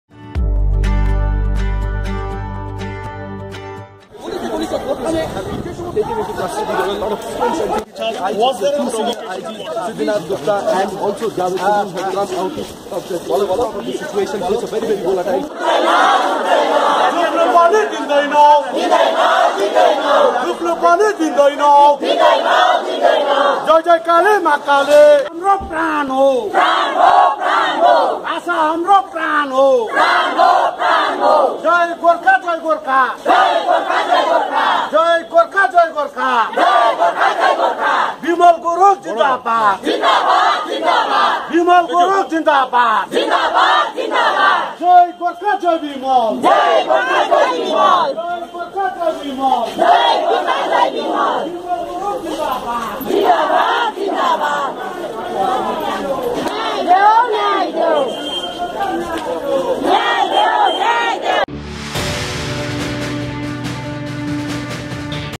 Darjeeling Bandh Supporters Assemble At Darjeeling Taxi Stand
The Gorkha Janmukti Morcha supporters, who are on an indefinite bandh in support of a separate Gorkhaland, Tuesday pelted stones at the police as they tried to stop them from enforcing their bandh in Darjeeling .